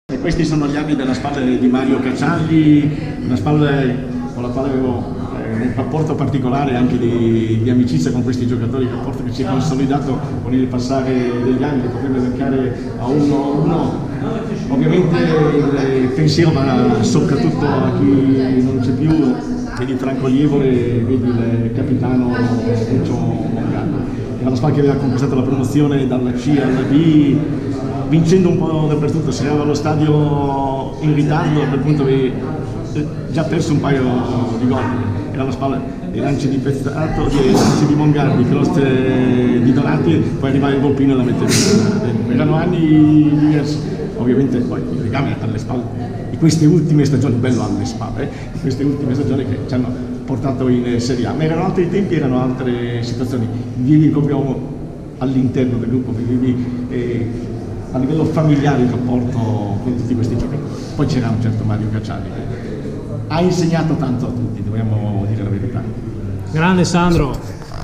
Alessandro Sovrani: la figurina della SPAL 1973-1974
Alessandro Sovrani di Telestense ci spiega in un minuto perché le figurine della stagione 73-74 della SPAL sono le sue preferite.

Video realizzato a margine dell'esposizione "l mito Spal attraverso le figurine Panini" allestita nel municipio di Ferrara